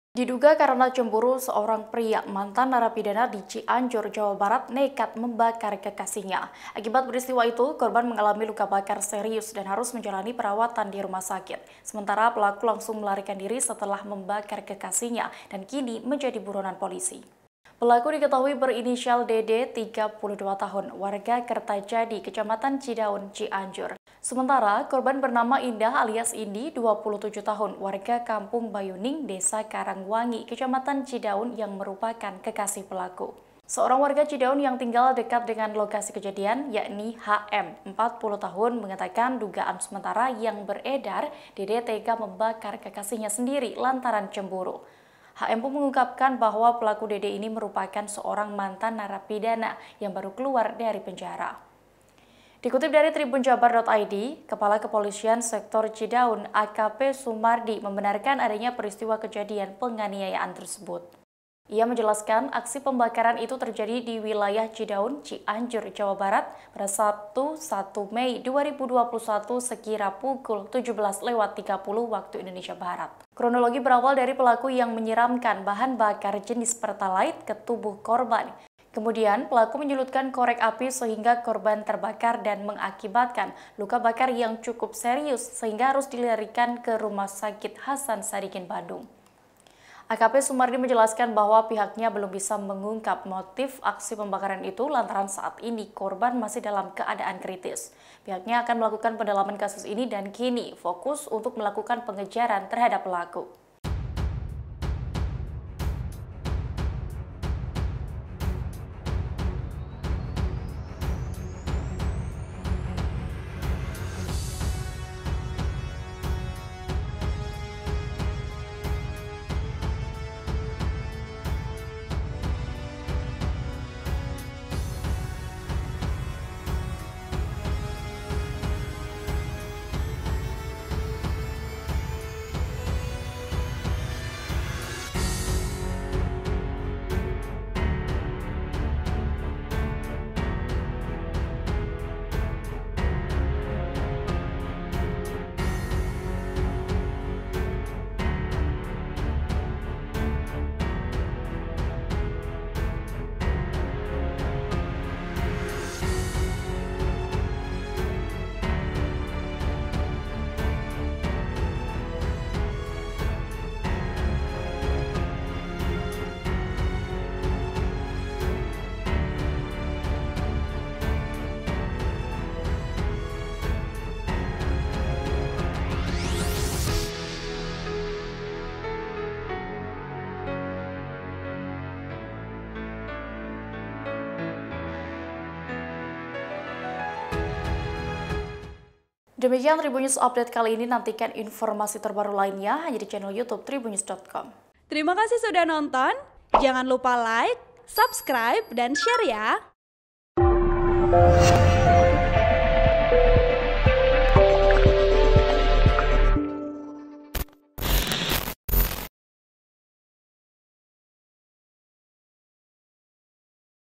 Diduga Motif Asmara, Pria Mantan Narapidana di Cianjur Siram Pertalite lalu Bakar Kekasihnya
TRIBUN-VIDEO.COM - Diduga karena cemburu, seorang pria mantan narapidana di Cianjur, Jawa Barat nekat membakar kekasihnya.

Akibat peristiwa itu, korban mengalami luka bakar serius dan harus menjalani perawatan di rumah sakit.

Sementara pelaku langsung melarikan diri setelah membakar kekasihnya dan kini menjadi buronan polisi.

Pelaku diketahui pria berinisial DD (32) warga Kertajadi, Kecamatan Cidaun, Cianjur.

Sementara korban bernama Indah alias Indi (27) warga Kampung Bayuning, Desa Karangwangi, Kecamatan Cidaun, yang merupakan kekasih pelaku.

Seorang warga Cidaun yang tinggal dekat dengan lokasi kejadian, HM (40), mengatakan dugaan sementara yang beredar DD tega membakar kekasih sendiri lantaran cemburu.

"Kalau di masyarakat isu beredar karena cemburu," ujar HM melalui sambungan telepon malam ini.

HM pun mengungkapkan bahwa pelaku DD merupakan seorang mantan narapidana yang baru keluar dari penjara.

"Kira-kira setahun baru keluar dari penjara," katanya.

Dikutip dari TribunJabar.id, Kepala Kepolisian Sektor Cidaun AKP Sumardi membenarkan adanya peristiwa kejadian penganiayaan tersebut.

"Benar peristiwa tersebut terjadi di halaman rumah salah seorang warga dan korban mengalami luka bakar cukup serius sehingga kritis dan dilarikan ke rumah sakit hasan Sadikin Bandung," ujar AKP Sumardi SH melalui sambungan telepon.

Ia menjelaskan, aksi pembakaran itu terjadi di wilayah Cidaun, Cianjur, Jawa Barat, Sabtu (1/5/2021) sekitar pukul 17.30 WIB.

Kronologi berawal dari pelaku menyiramkan bahan bakar jenis pertalite ke tubuh korban.

Kemudian pelaku menyulutkan korek api sehingga korban terbakar dan mengakibatkan luka bakar yang cukup serius sehingga harus dilarikan ke rumah sakit Hasan Sadikin Bandung.

AKP Sumardi menjelaskan bahwa pihaknya belum bisa mengungkap motif aksi pembakaran itu lantaran saat ini korban masih dalam keadaan kritis.

"Belum jelas apa motifnya sebab korban belum bisa dimintai keterangan karena keadaannya kritis," katanya.

Pihaknya akan melakukan pendalaman kasus dan kini fokus untuk melakukan pengejaran terhadap pelaku.

"Kami masih terus melakukan pendalaman dan mengumpulkan alat bukti serta meminta keterangan dari para saksi saksi dan mengejar pelaku untuk segera ditangkap guna untuk mempertanggungjawabkan perbuatannya," katanya. (Tribun-video.com/TribunJabar.id)

Artikel ini telah tayang di TribunJabar.id dengan judul Kabar yang Beredar di Cidaun Cianjur, Pemuda Tega Membakar Kekasih Diduga karena Cemburu,
Penulis: Ferri Amiril Mukminin
Editor: Tarsisius Sutom